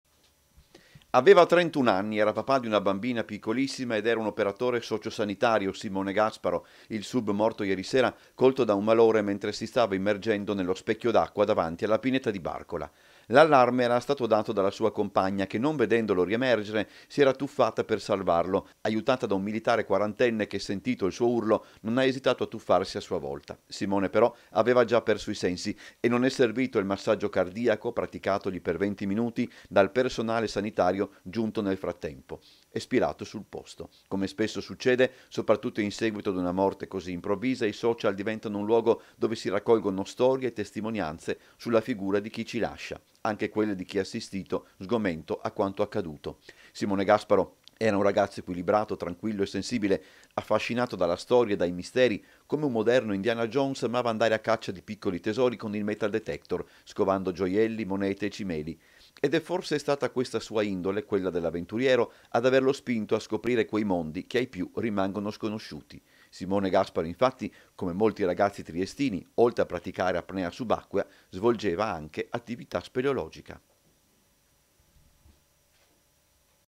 ADDIO SIMONE, L’INDIANA JONES CHE AMAVA LA PROFONDITA’ | 03/03/2021
NEWS 03/03/2021 - TRIESTE - ADDIO SIMONE, L’INDIANA JONES CHE AMAVA LA PROFONDITA’ || Aveva 31 anni, era papà di una bambina piccolissima, ed era un operatore socio-sanitario, Simone Gasparo, il sub morto ieri sera, colto da un malore mentre si stava immergendo nello specchio d’acqua davanti alla pineta di Barcola.
L’allarme era stato dato dalla sua compagna che, non vedendolo riemergere, si era tuffata per salvarlo aiutata da un militare quarantenne che, sentito il suo urlo, non ha esitato a tuffarsi a sua volta. Simone, però, aveva già perso i sensi. E non è servito il massaggio cardiaco, praticatogli per venti minuti dal personale sanitario giunto nel frattempo: è spirato sul posto.
Come spesso succede, soprattutto in seguito ad una morte così improvvisa, i social diventano un luogo dove si raccolgono storie e testimonianze sulla figura di chi ci lascia. Anche quelle di chi ha assistito, sgomento, a quanto accaduto.
Simone Gasparo era ragazzo equilibrato, tranquillo e sensibile. Affascinato dalla storia e dai misteri, come un moderno Indiana Jones, amava andare a caccia di piccoli tesori con il metal detector, scovando gioielli, monete e cimeli. Ed è forse stata questa sua indole, quella dell'avventuriero, ad averlo spinto a scoprire quei mondi che ai più rimangono sconosciuti. Simone infatti, come molti ragazzi triestini, oltre a praticare apnea subacquea, svolgeva anche attività speleologica. (Servizio di Umberto Bosazzi) - Segui Telequattro anche sul digitale terrestre! Visita il sito